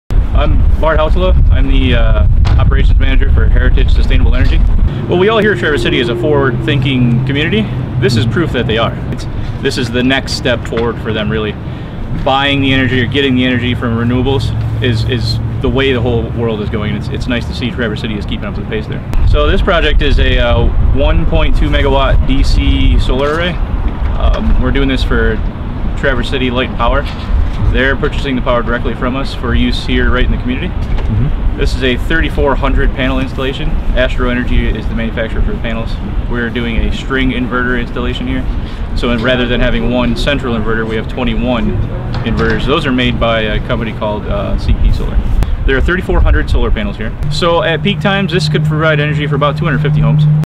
Introducing Traverse City Light & Power's Solar Panels
Bart Hautala, operations manager for Heritage Sustainable Energy, introduces the new solar array next to the windmill on M-72 just west of Traverse City. This array—the result of a collaboration between Heritage and the city-owned utility Traverse City Light & Power—represents a huge step in Traverse City's push toward renewable energy.